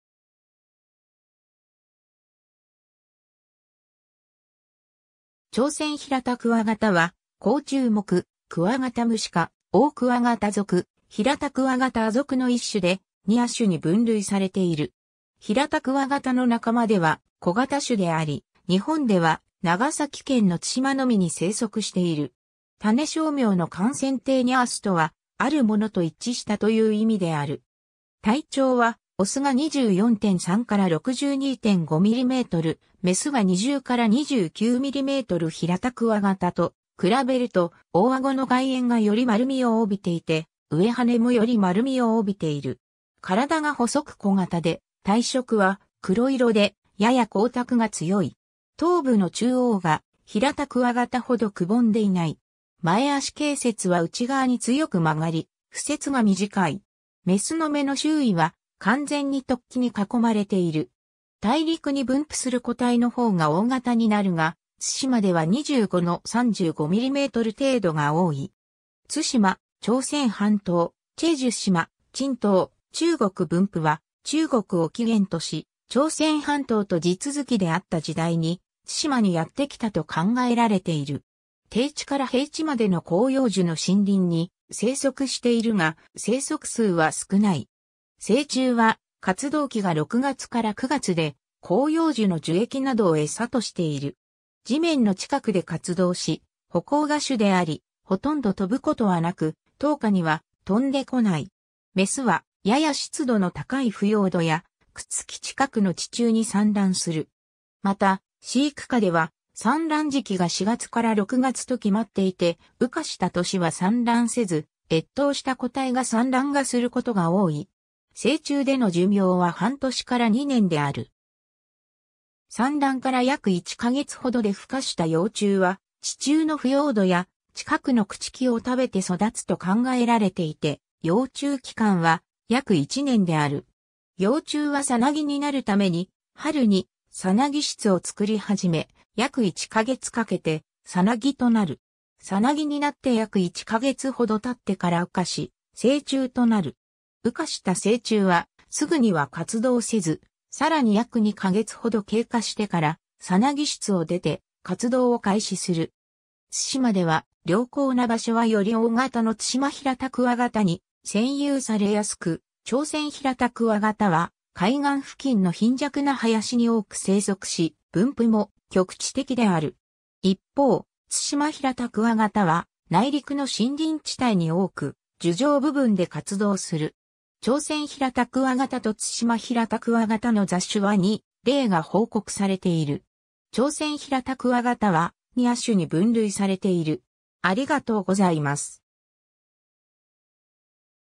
チョウセンヒラタクワガタ
チョウセンヒラタクワガタ（朝鮮平鍬形、Dorcus consentaneus）は、コウチュウ目・クワガタムシ科・オオクワガタ属・ヒラタクワガタ亜属の1種で、2亜種に分類されている。
 ヒラタクワガタの仲間では小型種であり、日本では長崎県の対馬のみに生息している。
 種小名のconsentaneusとは「あるものと一致した」という意味である。
 体長はオスが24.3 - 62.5mm、メスが20 - 29mm ヒラタクワガタと比べると、大アゴの外縁が より丸みを帯びていて、上翅（じょうし）も より丸みを帯びている。
 体が細く小型で、体色は黒色で やや光沢が強い。
 頭部の中央がヒラタクワガタほど窪んでいない。
 前足脛節は内側に強く曲り、付節（爪を含む短い節が連続している部分）が短い。
 メスの眼の周囲は完全に突起に囲まれている。
 大陸に分布する個体のほうが大型になるが、対馬では25 - 35mm程度が多い。
 対馬・朝鮮半島・済州島・珍島・中国 分布は、中国を起源とし、朝鮮半島と地続きであった時代に対馬にやってきたと考えられている。
 低地から平地までの広葉樹の森林に生息しているが、生息数は少ない。
成虫は、活動期が6月から9月で、広葉樹の樹液などをエサとしている。
地面の近くで活動し、歩行が主であり、ほとんど飛ぶことはなく、灯火（明かり）には飛んで来ない。
メスは、やや湿度の高い腐葉土や、朽木近くの地中に産卵する。
また、飼育下では、産卵時期が4月から6月と決まっていて、羽化した年は産卵せず、越冬した個体が産卵がすることが多い。
成虫での寿命は半年から2年である。
 産卵から約1か月ほどで孵化した幼虫は、地中の腐葉土や、近くの朽木を食べて育つと考えられていて、幼虫期間は約1年である。
幼虫は蛹になるために、春に蛹室（ようしつ）を作り始め、約1か月かけて蛹となる。
蛹になって約1か月ほど経ってから羽化し、成虫となる。
羽化した成虫は、すぐには活動せず、さらに約2か月ほど経過してから、蛹室を出て活動を開始する。
 対馬では、良好な場所はより大型のツシマヒラタクワガタに占有されやすく、チョウセンヒラタクワガタは海岸付近の貧弱な林に多く生息し、分布も局地的である。
一方、ツシマヒラタクワガタは内陸の森林地帯に多く、樹上部分で活動する。
チョウセンヒラタクワガタとツシマヒラタクワガタの雑種は2例が報告されている。
 チョウセンヒラタクワガタは、2亜種に分類されている。